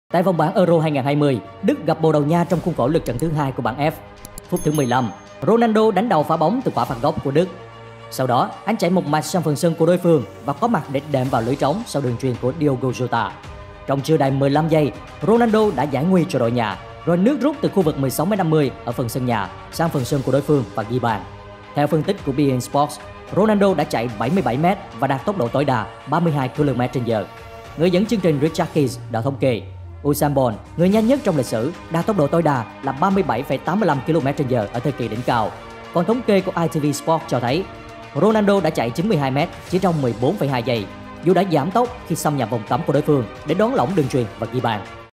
Ronaldo, ăn gì mà khỏe vậy? | Vũ trụ Man United | Shorts
Tại vòng bảng Euro 2020, Đức gặp Bồ Đào Nha trong khuôn khổ lượt trận thứ hai của bảng F. Phút 15, Ronaldo đánh đầu phá bóng từ quả phạt góc của Đức. Anh, sau đó, chạy một mạch sang phần sân đối phương và có mặt để đệm vào lưới trống sau đường chuyền của Diogo Jota. Trong chưa đầy 15 giây, Ronaldo giải nguy cho đội nhà, rồi nước rút từ khu vực 16m50 ở sân nhà sang sân đối phương để ghi bàn. Theo phân tích của beIN Sports, Ronaldo chạy 97m và đạt tốc độ tối đa 32km/h. Người dẫn chương trình Richard Keys thống kê, Usain Bolt - người nhanh nhất trong lịch sử - đạt tốc độ tối đa 37,85km/giờ ở thời kỳ đỉnh cao. Còn thống kê của ITV Sport cho thấy, Ronaldo chạy 92m chỉ trong 14,2 giây, dù đã giảm tốc khi xâm nhập vòng cấm đối phương để đón lõng đường chuyền và ghi bàn.

Cần nhớ rằng Ronaldo thực hiện pha bóng trên ở tuổi 36 - độ tuổi mà nhiều đồng nghiệp của anh hoặc đã giải nghệ, hoặc chuyển sang chơi bóng theo kiểu “dưỡng già” ở các giải đấu ít sự cạnh tranh hơn, trong khi cầu thủ người Bồ Đào Nha thì vẫn là chủ công của đội tuyển quốc gia và tại các CLB mà anh thi đấu: Juventus và Manchester United – những đội bóng đang chơi tại các giải đấu đầy khắc nghiệt như Serie A và Ngoại Hạng Anh. Vậy đâu là bí quyết để Ronaldo có được nền tảng thể lực sung mãn như thế và duy trì nó qua nhiều năm?